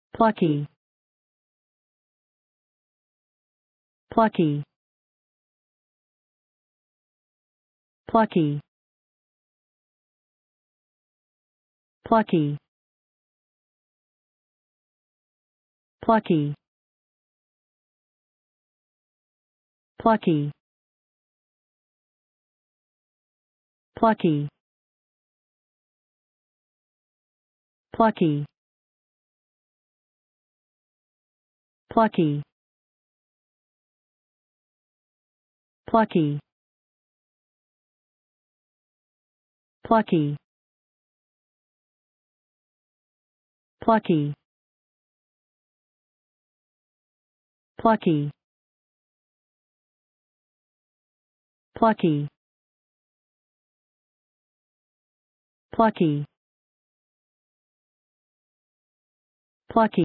Pronunciation of Plucky | Definition of Plucky
Plucky pronunciation | How to pronounce Plucky in English?
/`pləkiː/
Meaning of Plucky | What is Plucky?
(1) (adjective) marked by courage and determination in the face of difficulties or danger; robust and uninhibited; (2) (adjective) showing courage;
What is Plucky | Plucky meaning and definition?